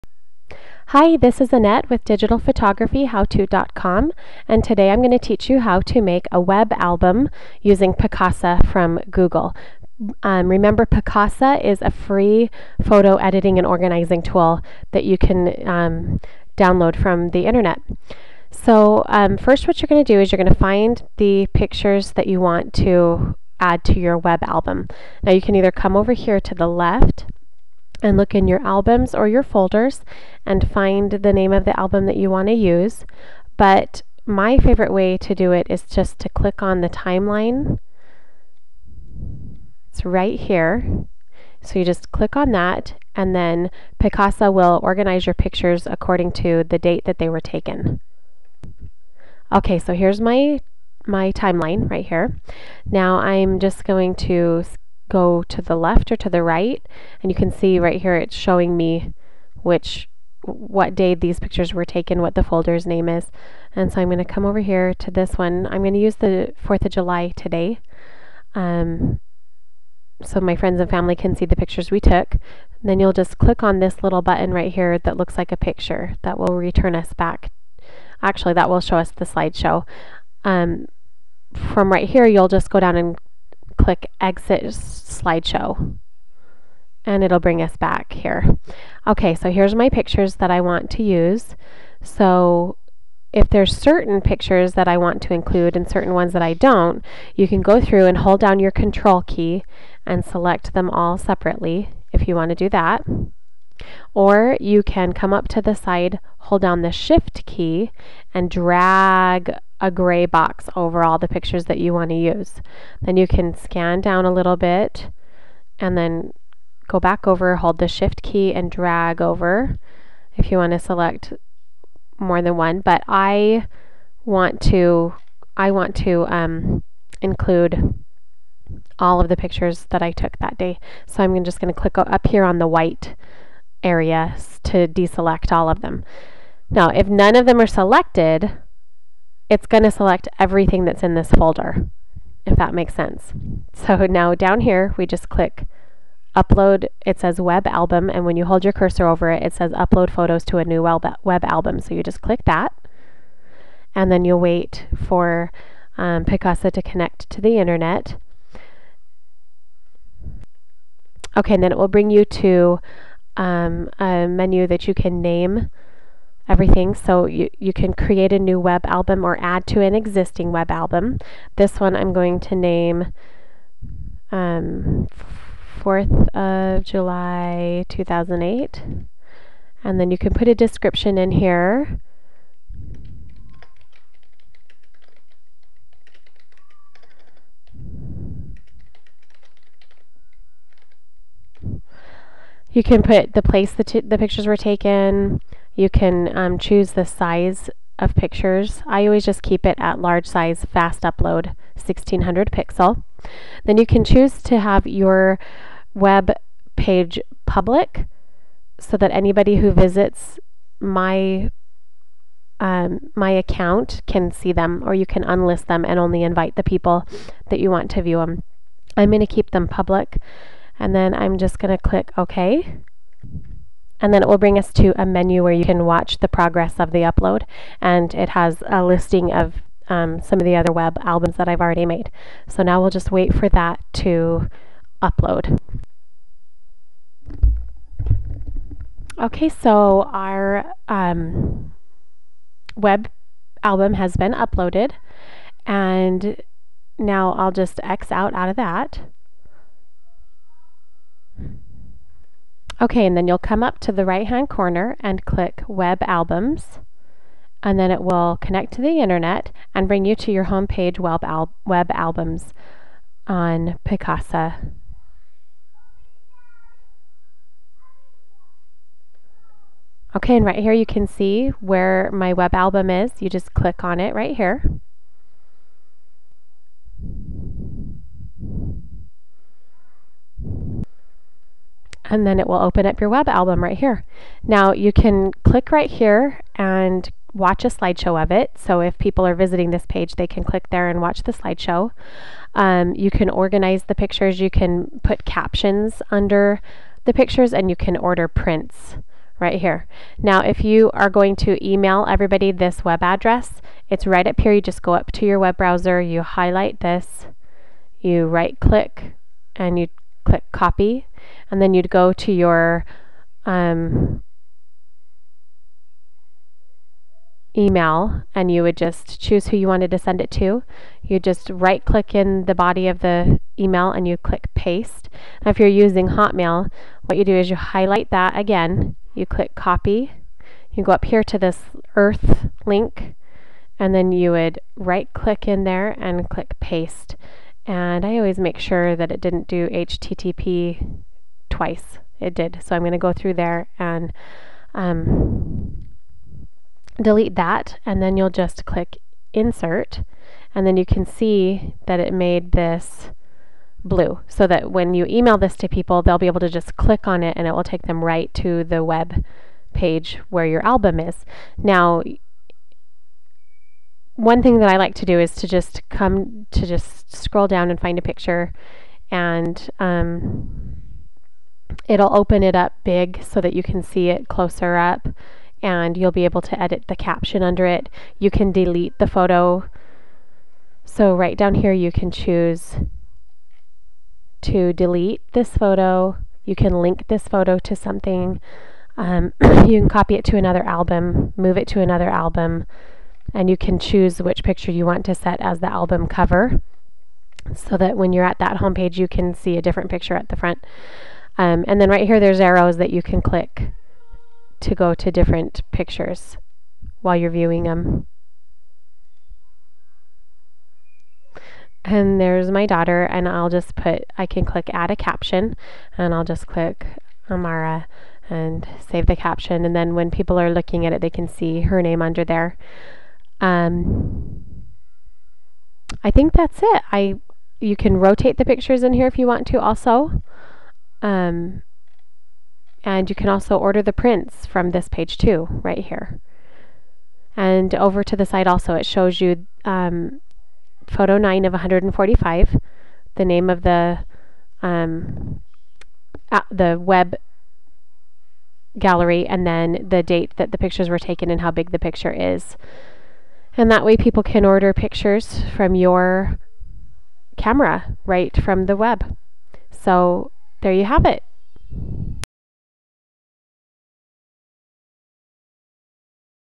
How to create a photo web album or gallery using Picasa
Learn how to create a web album (gallery) using Picasa, a free photo editor from google.